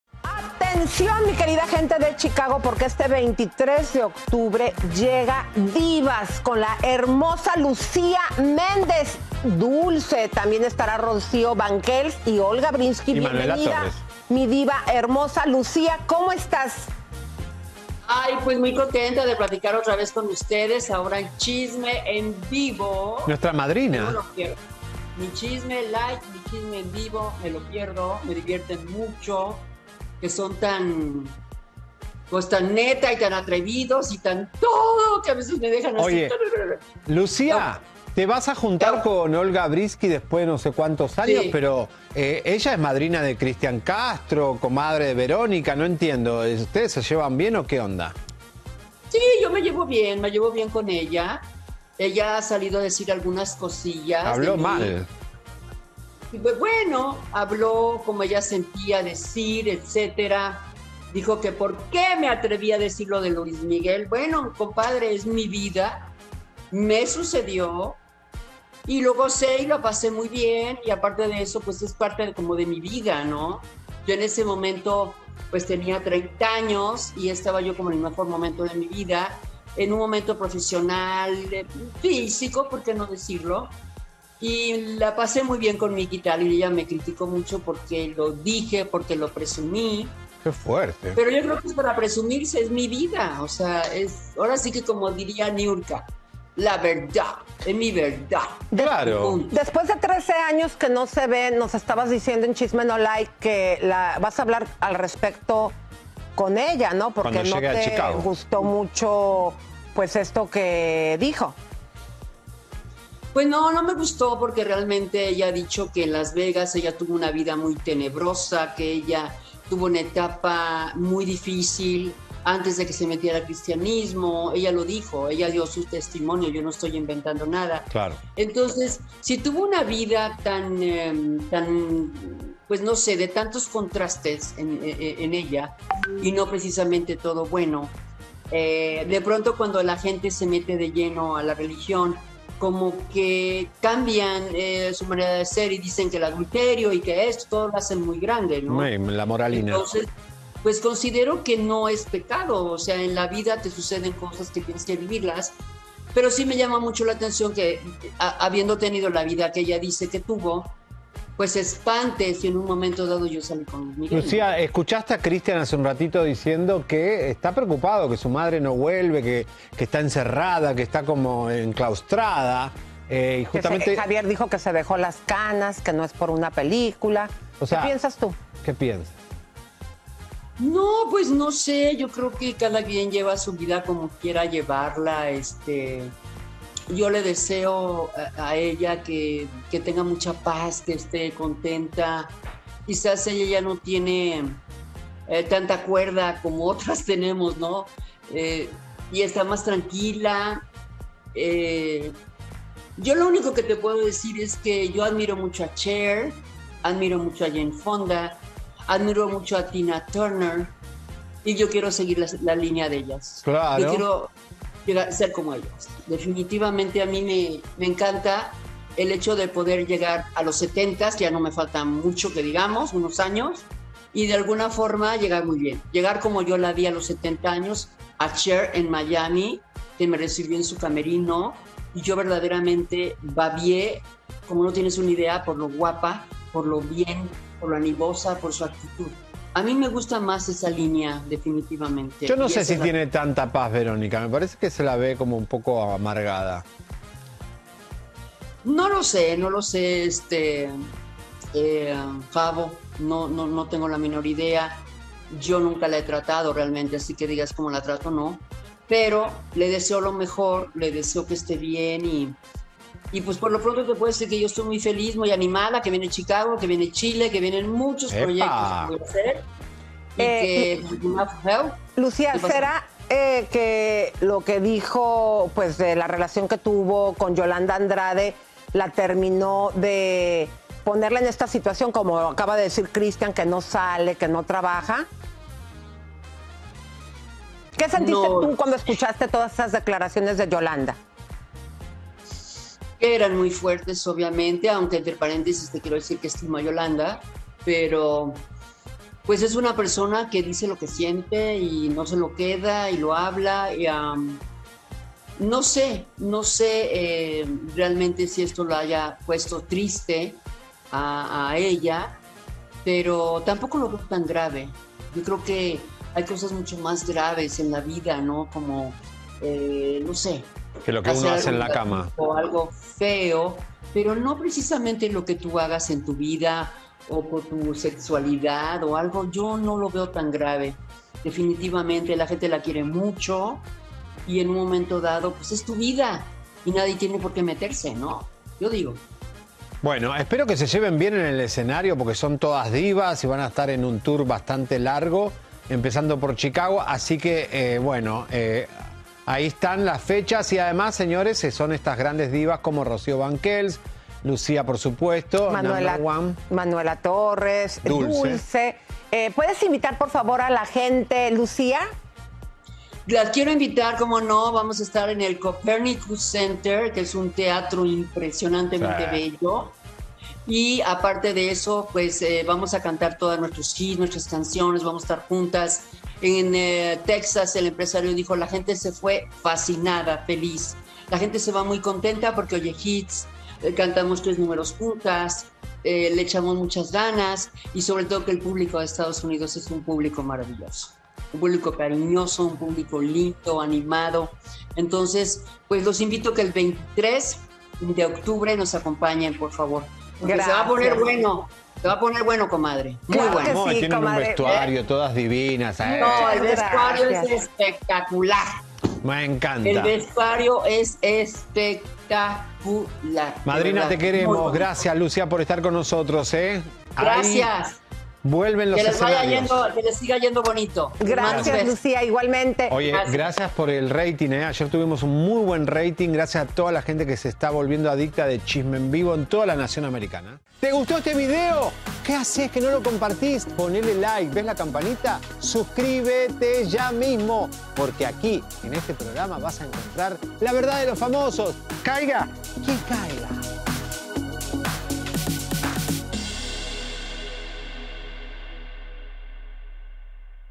LUCÍA MÉNDEZ se junta con puras divas para irse de GIRA 2021 | Chisme en Vivo
Lucía Méndez en entrevista nos explica en qué consiste su gira con las divas más grandes de todo México.